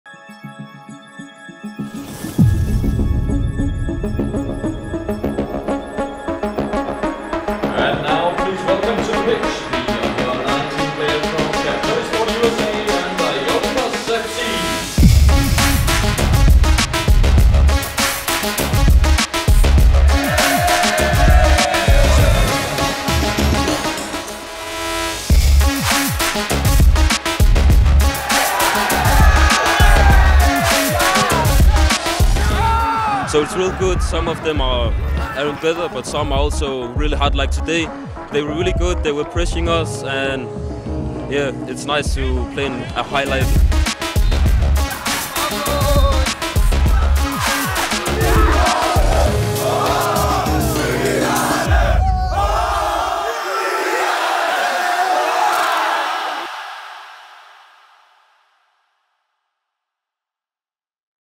Capelli Sport Cup Denmark 2022 Match Day 3
HB Køge player gives his thoughts on the competition at the Capelli Sport Cup.